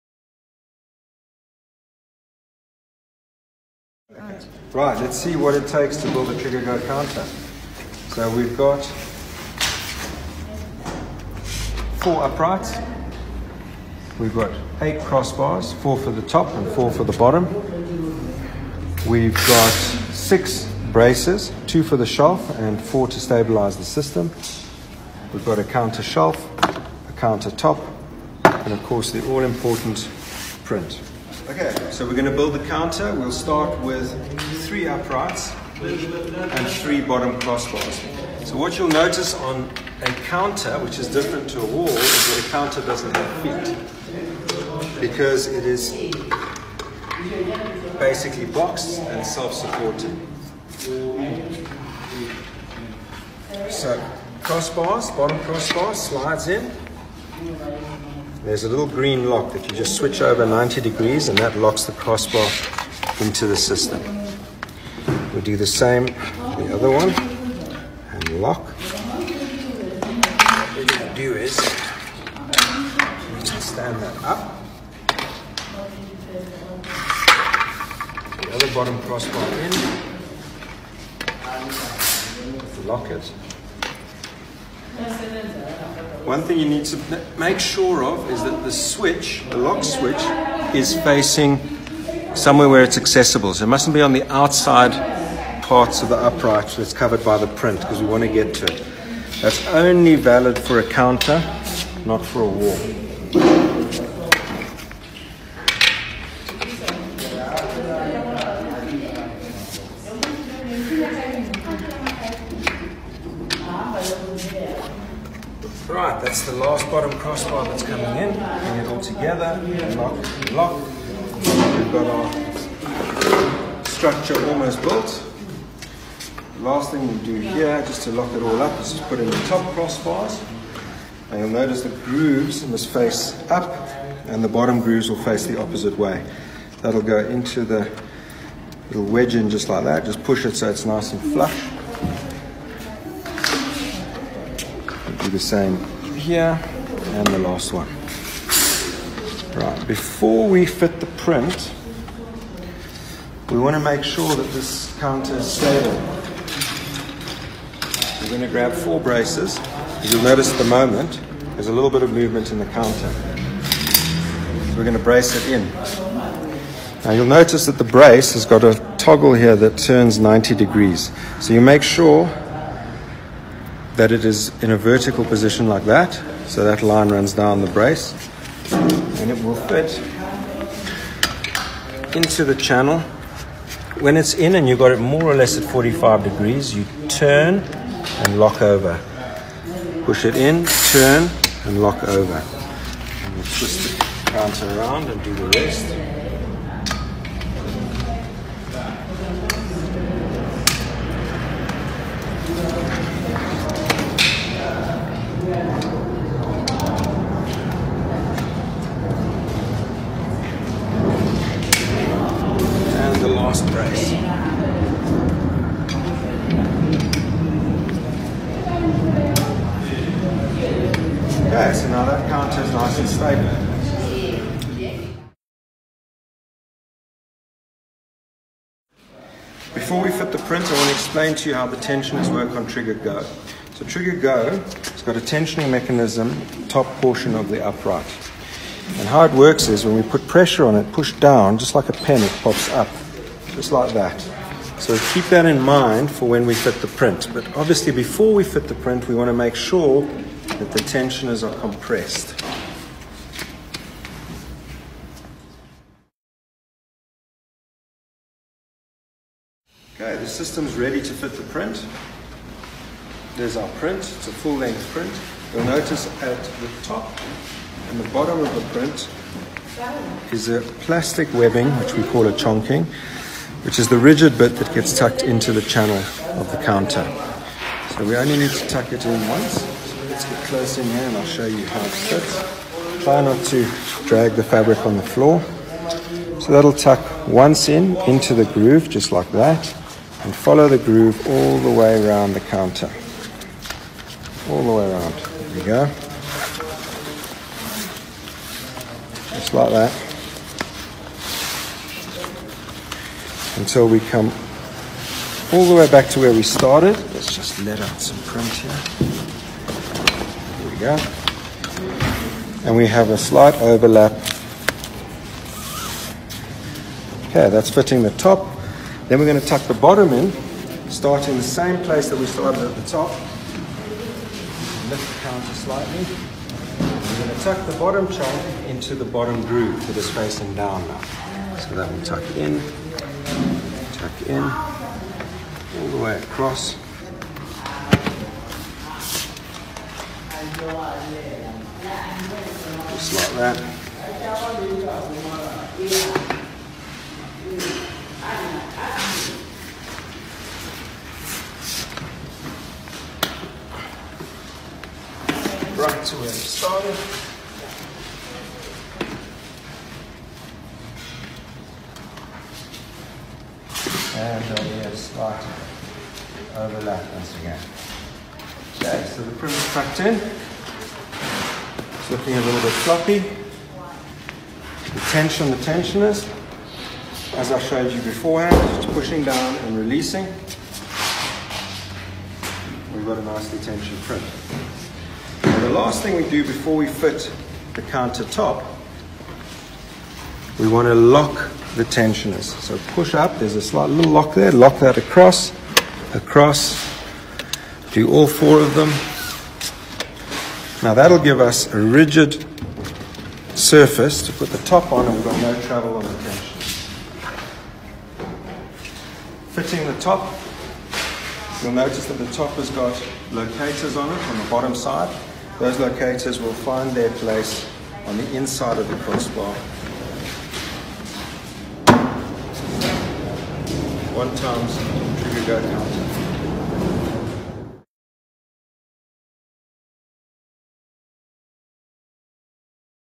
TRIGA Go Counter Build Demonstration
Sandro from Gawk Visual Engineers (African Master Distributor for TRIGA) demonstrates just how easy it is to build a counter using TRIGA Go.